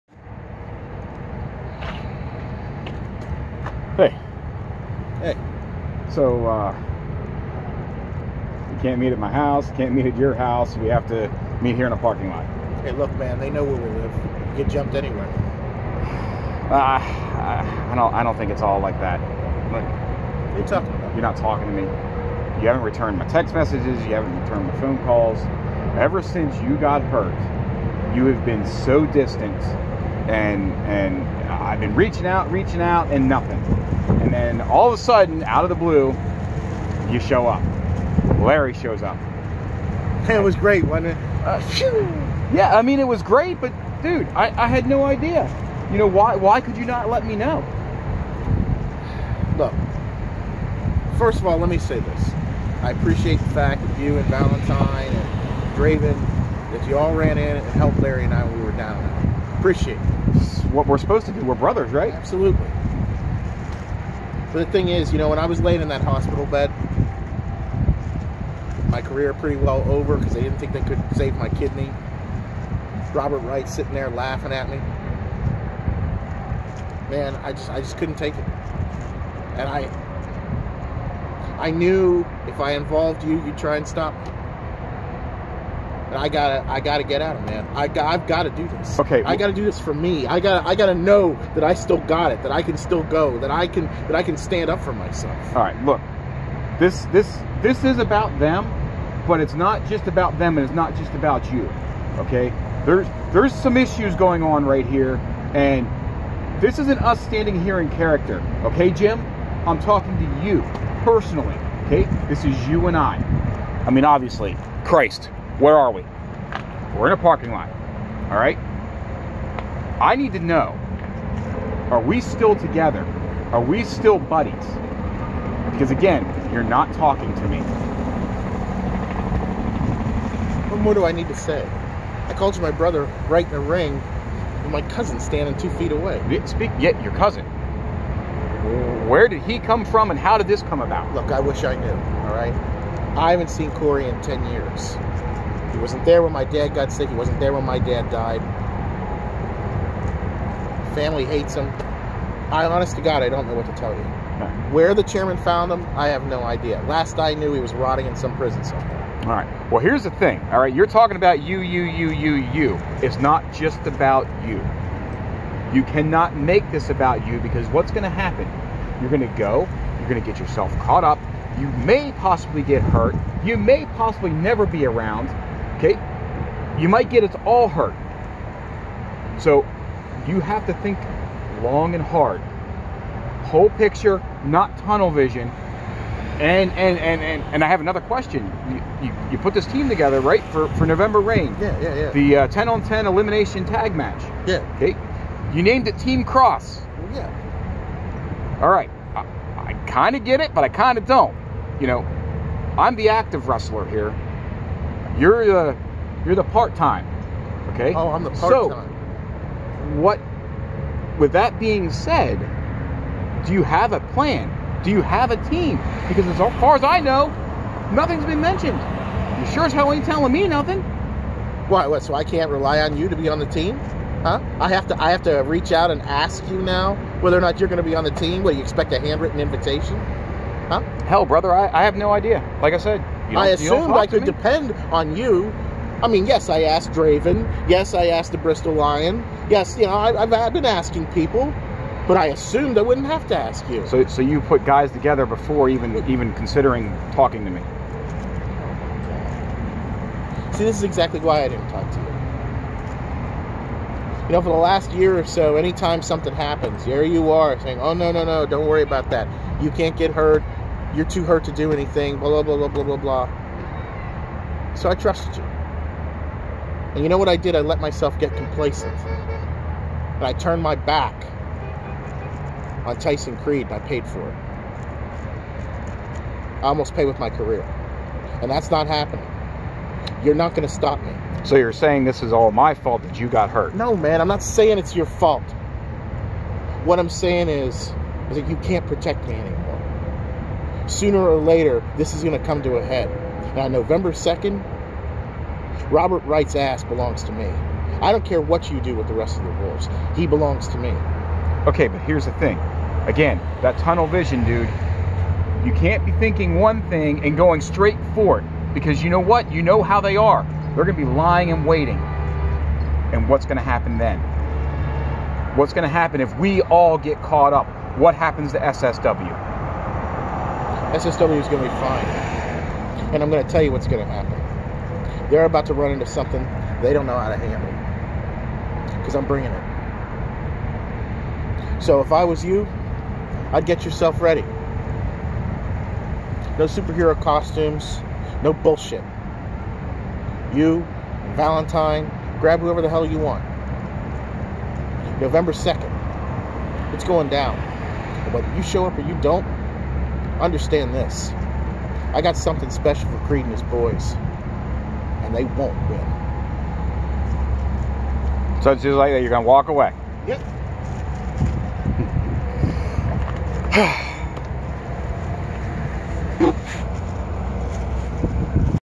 Is There Trouble Brewing ?
Inferno and Diamond Dex Cross meet in a public parking lot, and The SSW Cameras were there to capture this uncomfortable conversation.